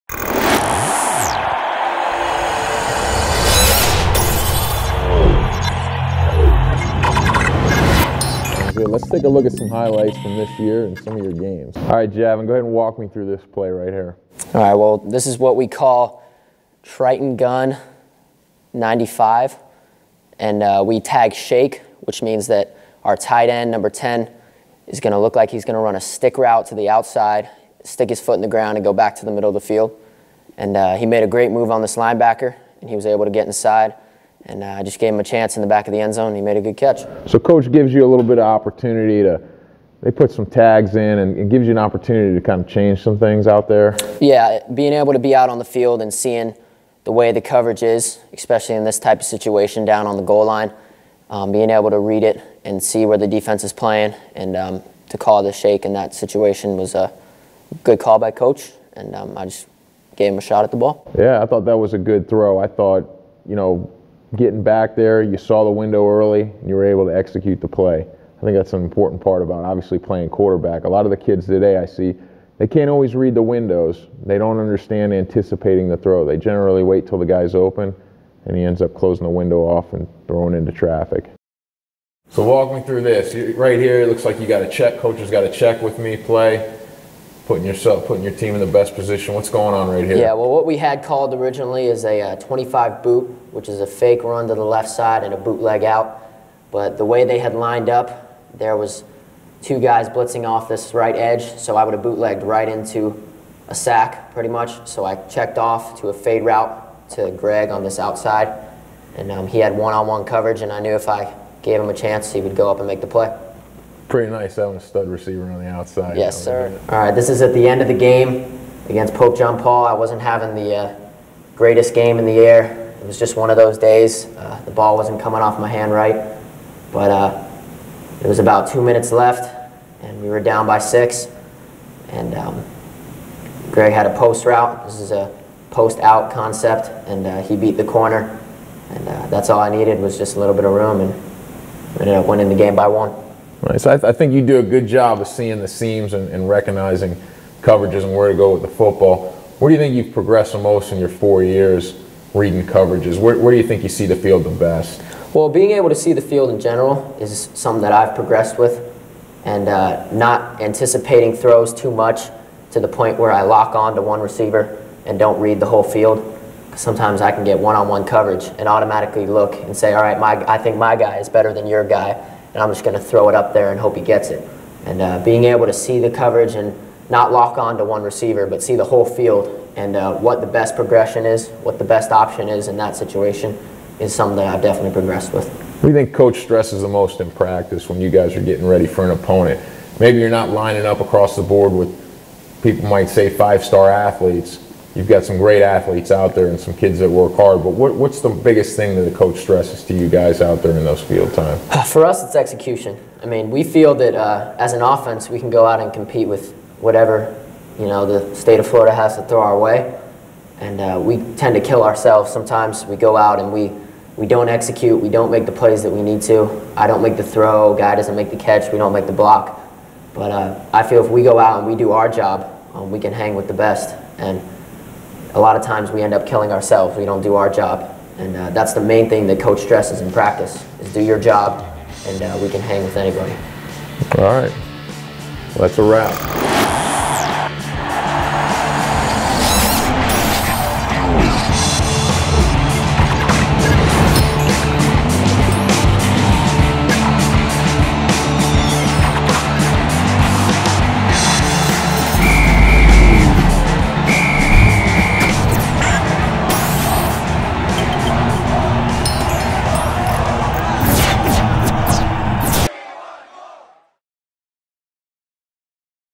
QB Lab-Javan Shashaty Pt. 3
Ken Mastrole talks with Javan Shashaty, quarterback from Calvary Christian Academy in Ft. Lauderdale